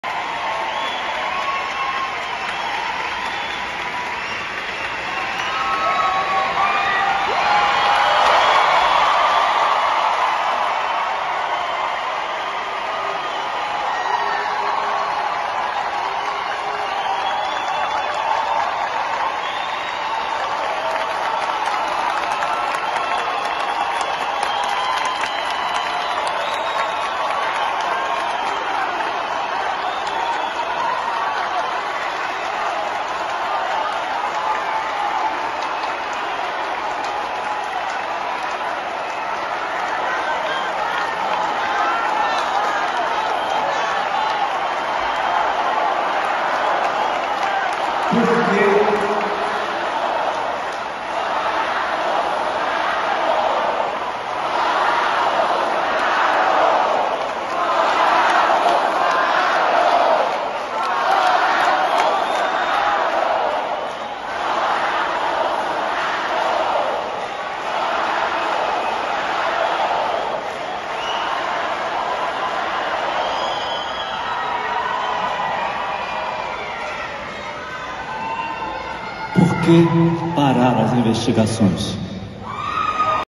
Caetano cobra investigações sobre crimes na Amazônia e puxa coro de ‘fora Bolsonaro’
Show de Caetano Veloso aconteceu em Brasília. Ele perguntou por que pararam as investigações sobre assassinatos do jornalista Dom Phillips e do indigenista Bruno Pereira